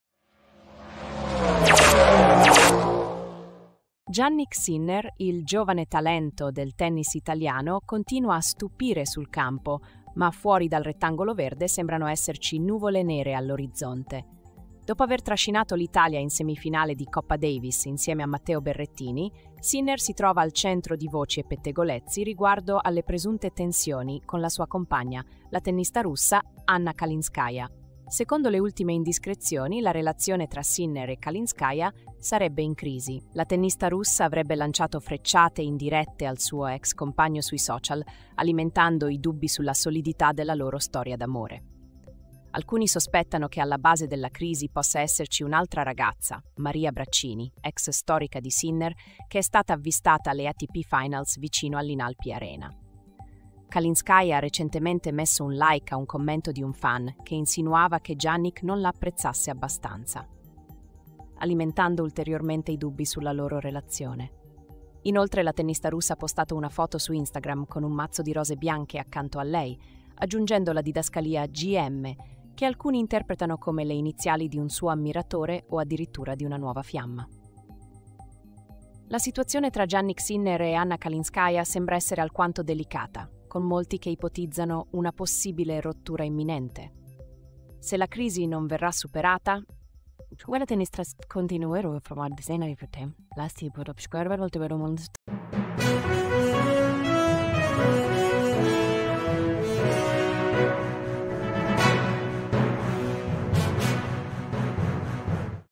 SCOPRI IL DRAMMA TRA JANNIK SINNER E ANNA KALINSKAYA: CRISI IN VISTA
Jannik Sinner, il giovane talento del tennis italiano, continua a stupire sul campo ma fuori dal rettangolo verde sembrano esserci nuvole nere all'orizzonte. Dopo aver trascinato l'Italia in semifinale di Coppa Davis insieme a Matteo Berrettini, Sinner si trova al centro di voci e pettegolezzi riguardo alle presunte tensioni con la sua compagna, la tennista russa Anna Kalinskaya.

Secondo le ultime indiscrezioni, la relazione tra Sinner e Kalinskaya sarebbe in crisi. La tennista russa avrebbe lanciato frecciate indirette al suo (ex?) compagno sui social, alimentando i dubbi sulla solidità della loro storia d'amore. Alcuni sospettano che alla base della crisi possa esserci un'altra ragazza, Maria Braccini, ex storica di Sinner, che è stata avvistata alle ATP Finals vicino all'Inalpi Arena.

Kalinskaya ha recentemente messo un like a un commento di un fan che insinuava che Jannik non la apprezzasse abbastanza, alimentando ulteriormente i dubbi sulla loro relazione. Inoltre, la tennista russa ha postato una foto su Instagram con un mazzo di rose bianche accanto a lei, aggiungendo la didascalia "GM", che alcuni interpretano come le iniziali di un suo ammiratore o addirittura di una nuova fiamma.

La situazione tra Jannik Sinner e Anna Kalinskaya sembra essere alquanto delicata, con molti che ipotizzano una possibile rottura imminente. Se la crisi non verrà superata, la tennista russa continuerà a far parlare di sé, lasciando aperte molte domande sul futuro della loro relazione.